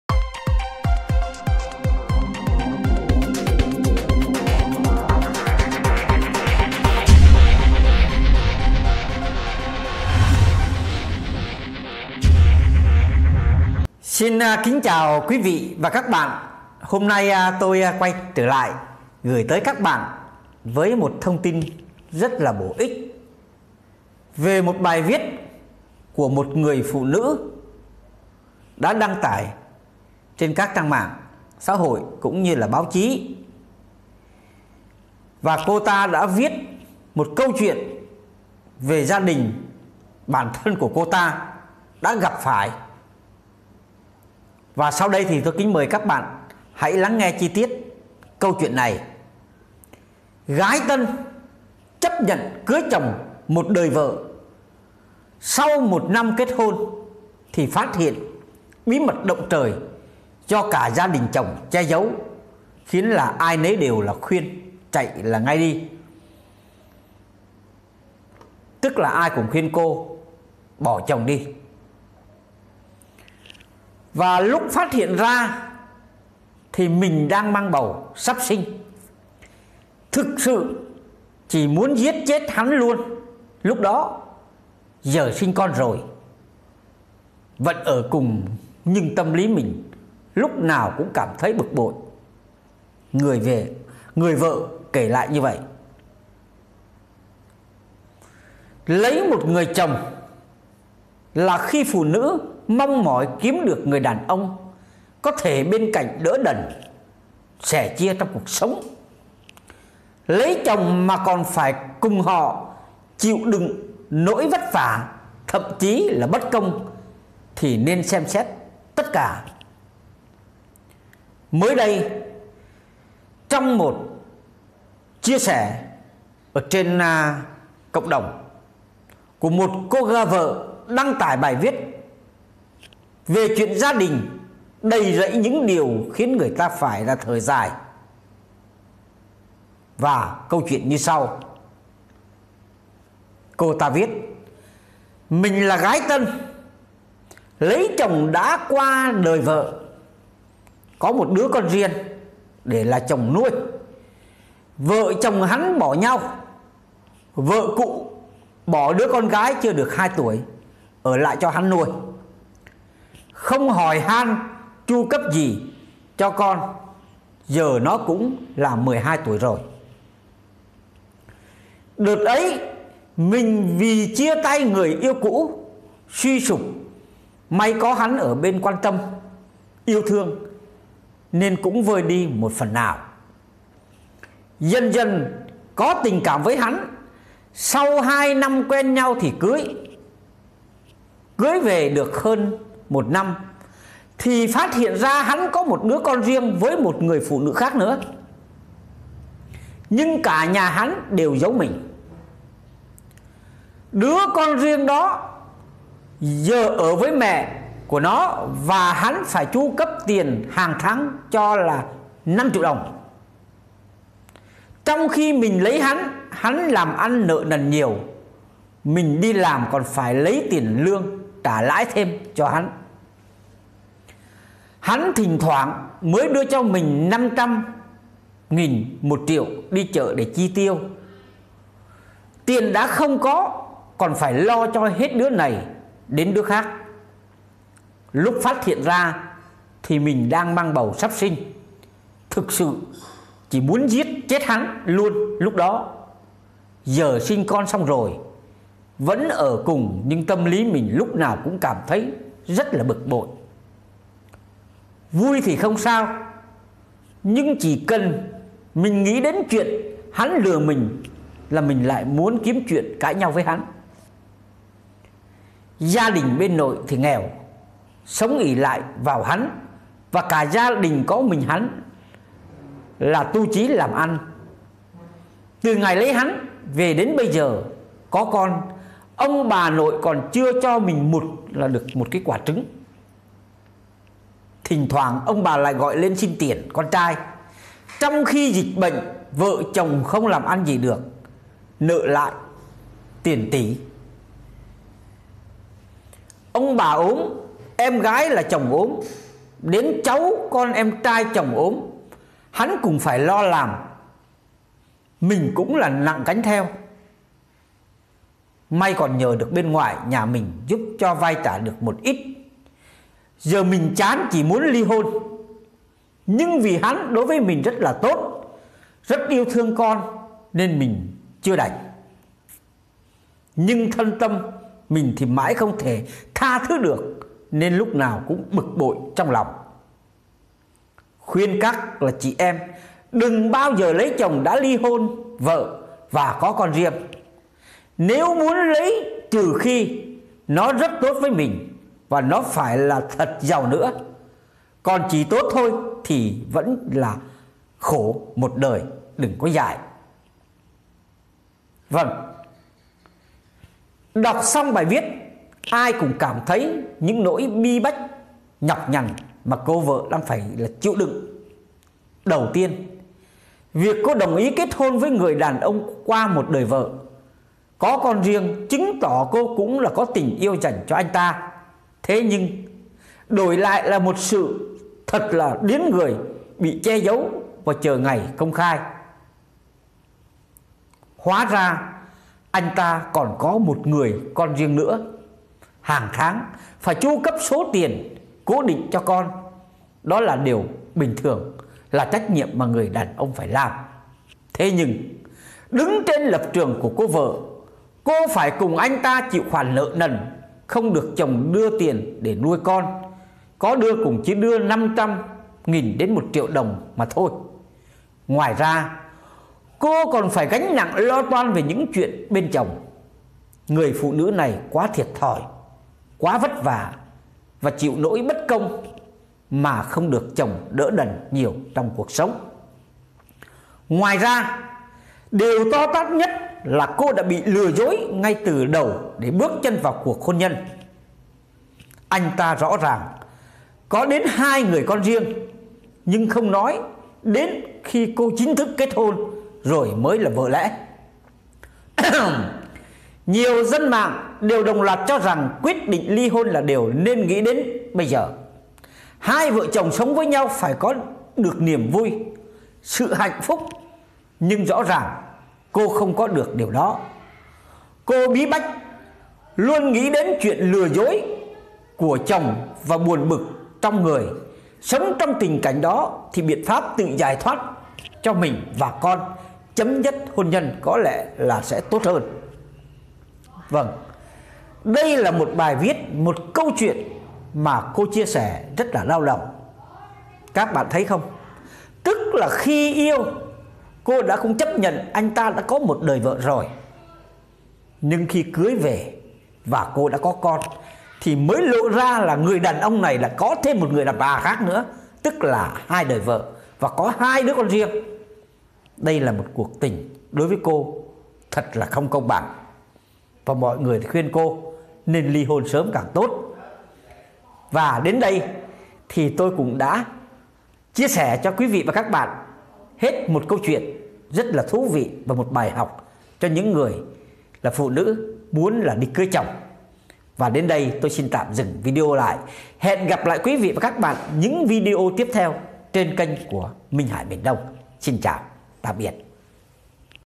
🔴 Nóng: Gái Tân Cưới Chồng 1 Đời Vợ. Sau Khi Kết Hôn Thì Phát Hiện Bí Mật Động Trời.
Hôm nay tôi gửi tới các bạn với một thông tin rất là thú vị, gái tân chấp nhận cưới chồng một đời vợ sau một năm kết hôn, thì phát hiện bí mật động trời do cả gia đình chồng che giấu khiến ai nấy đều khuyên chạy ngay đi.